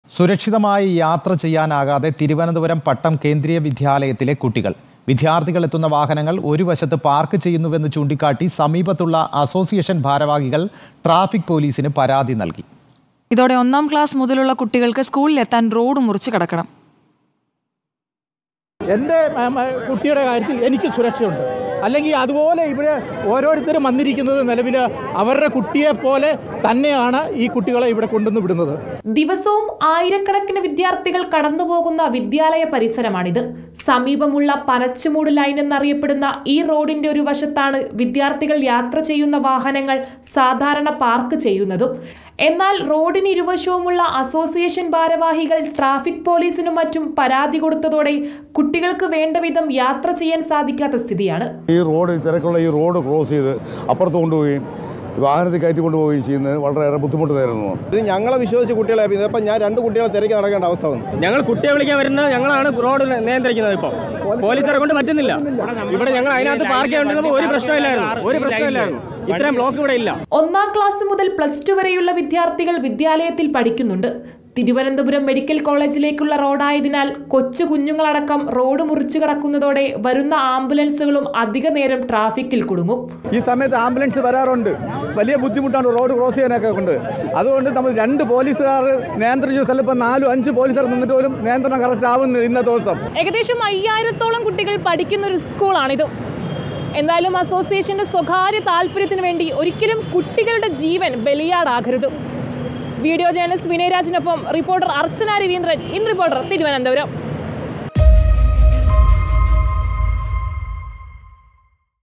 സുരക്ഷിതമായി യാത്ര ചെയ്യാനാകാതെ തിരുവനന്തപുരം പട്ടം കേന്ദ്രീയ വിദ്യാലയത്തിലെ കുട്ടികൾ | Trivandrum
With Regards
Team RBC